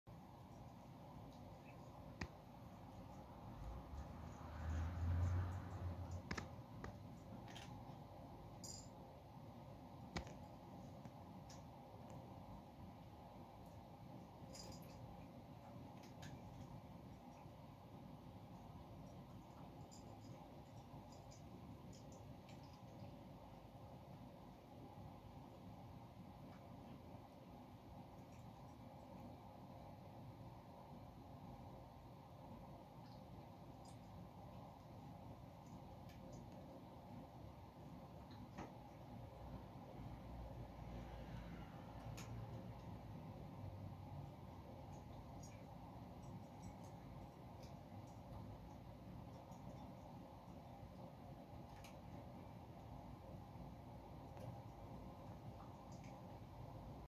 アメショー母娘の食事風景
母娘のご飯タイム、母猫はちょっと食べてすぐカリカリを放置し移動してしまう。
猫缶は即たべます(^_^)
カリカリは又他の場所でたべたがります。
巨大な娘猫は即完食、母猫が残したカリカリも即､隠さないと食べられてしまいます。